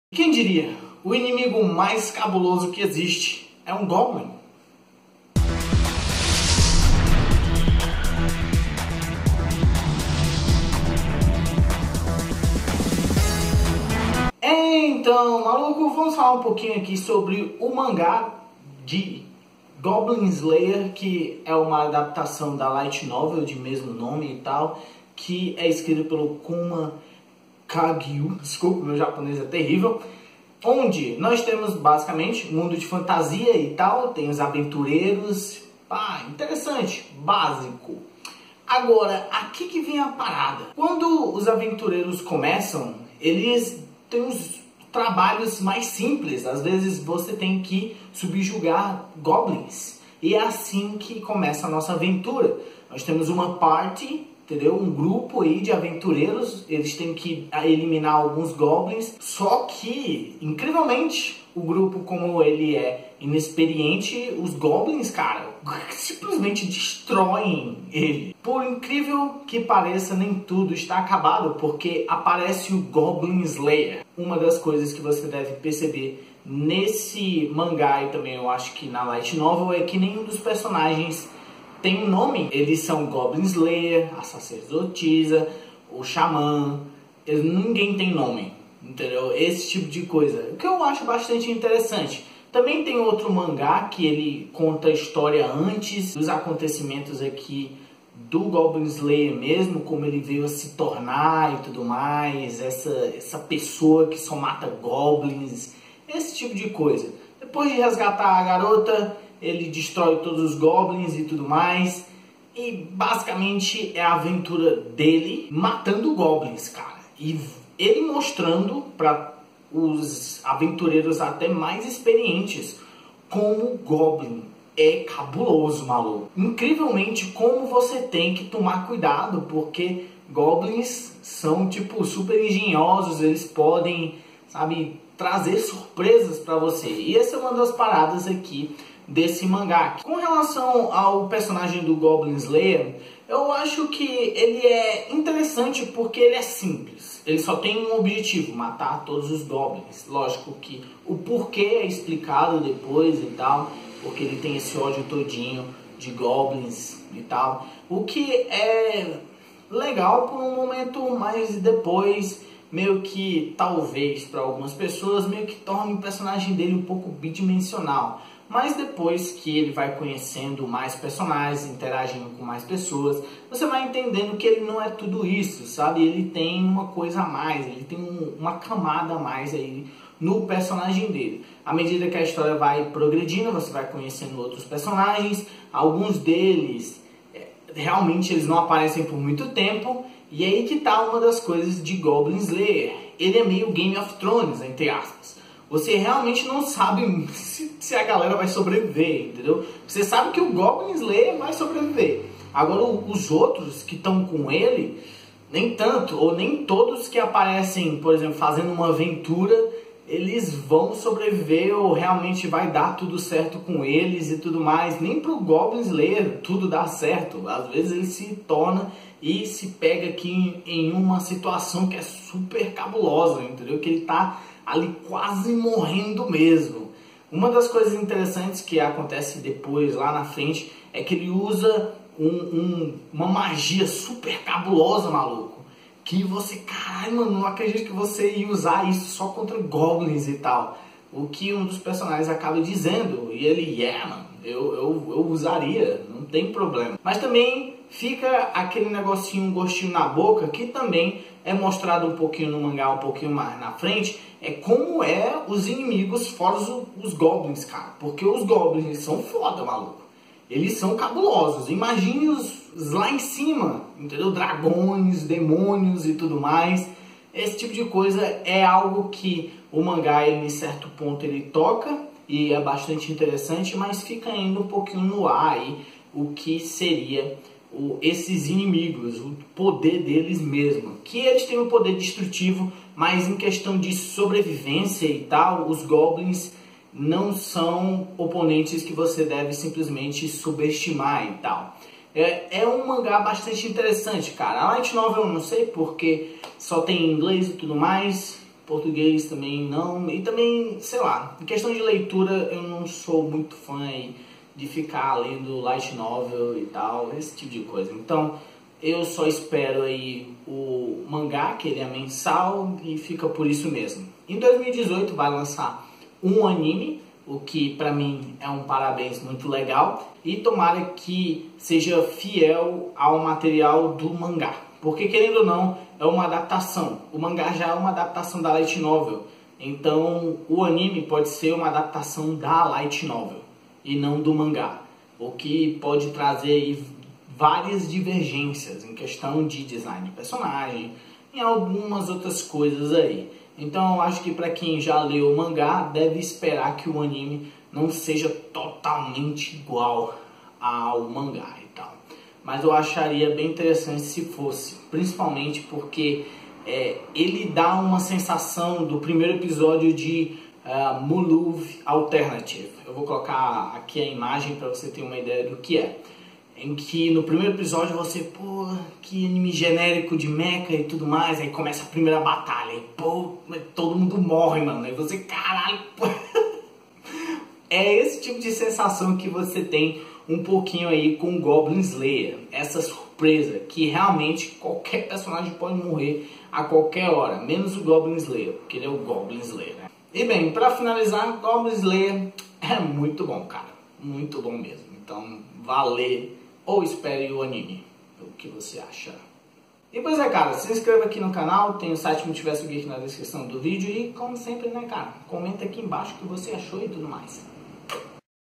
Goblin Slayer é Muito Bom | Crítica de Mangá
Goblin Slayer é muito bom,pelo menos o mangá que é a adaptação da light novel do Kumo Kagyu que conta a historia de um aventureiro que mata goblins,ah e vai ter anime em 2018